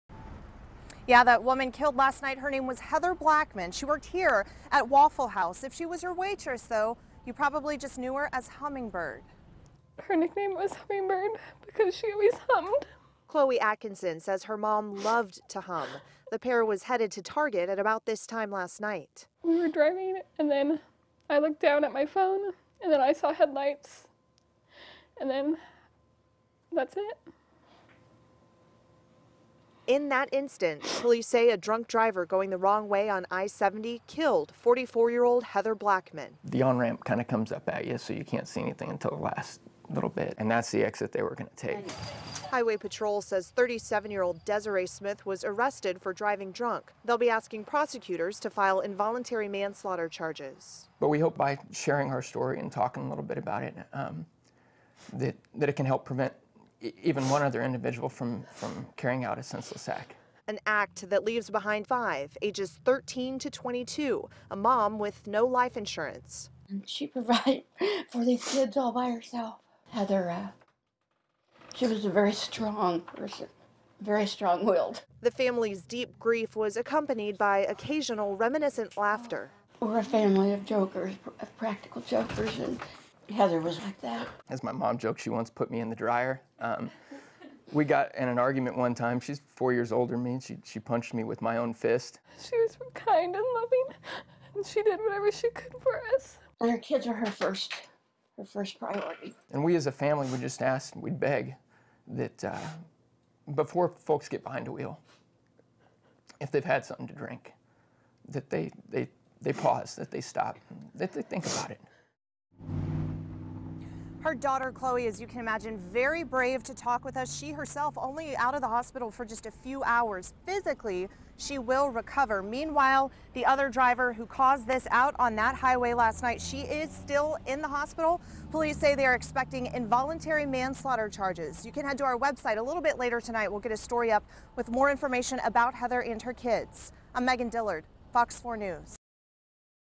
Metro mom killed by wrong-way driver
Less than 24 hours after her mother was killed in a wrong-way, head-on crash, the victim`s daughter is sharing their final moments together.